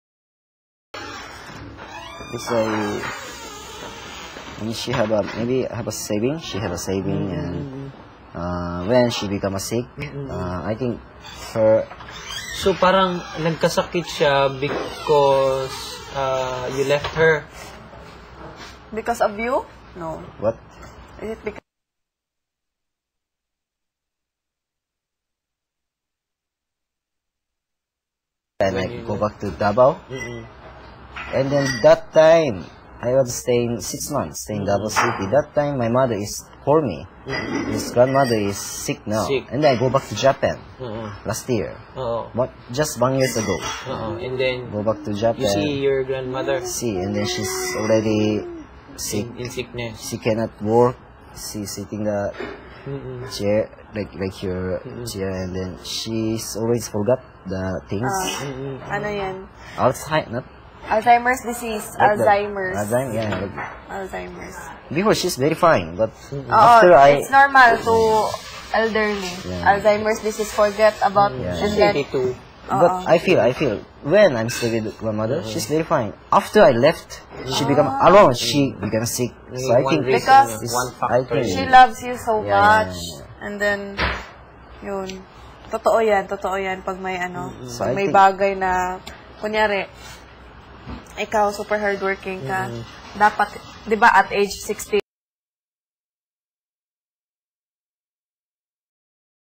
Seiichi Conversation With Steph & Naprey (Part 3) [12/22/11]
Alamin Ang Pinaguusapan Nila Seiichi, Naprey and Steph